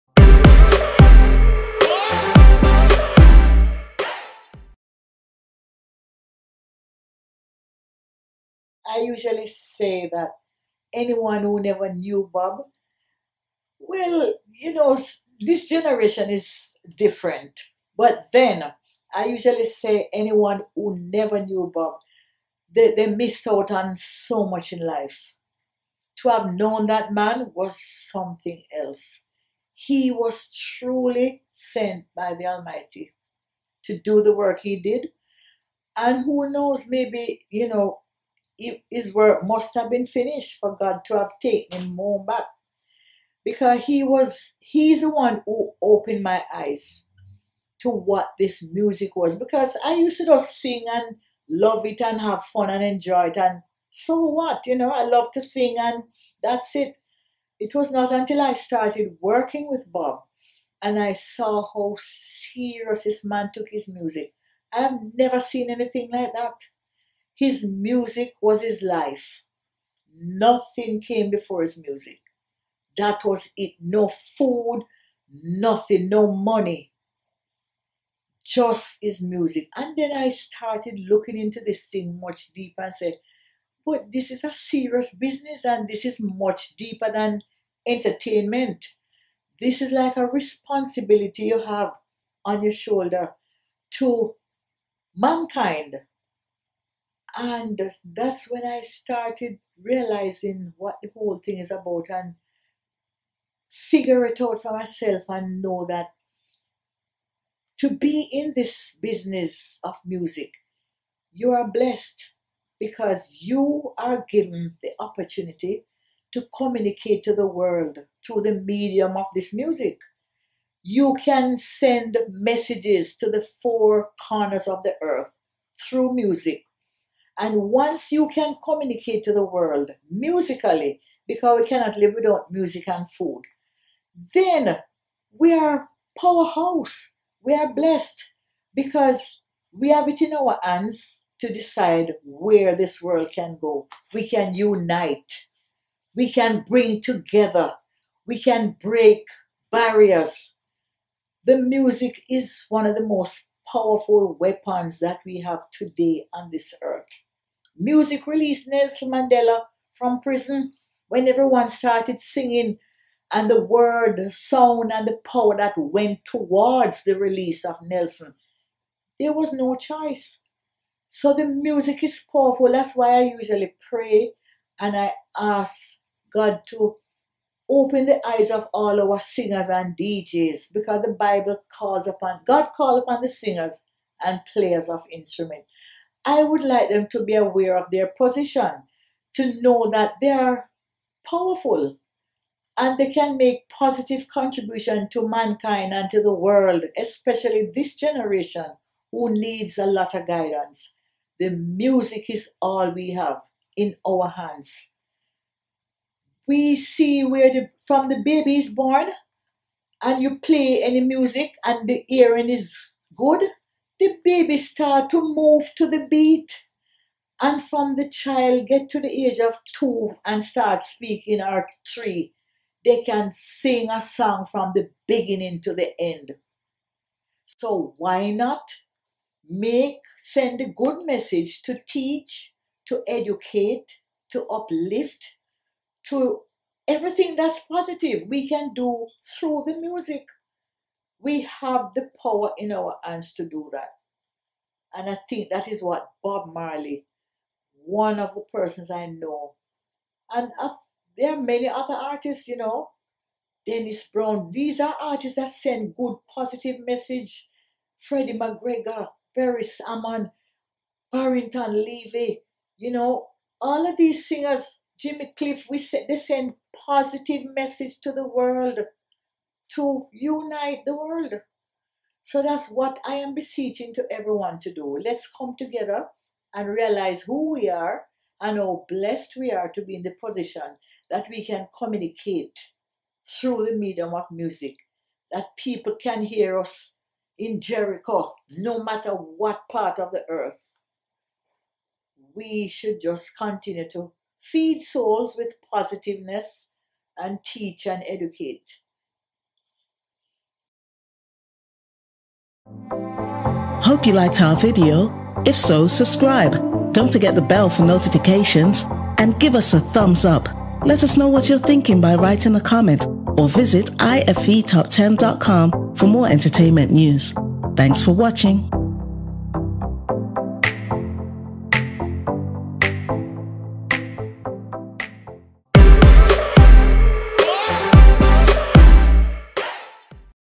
Marcia Griffiths on her friendship with Bob Marley
Camera bag
Camera lighting

Location: Vigilanti Studio, Kingston, Jamaica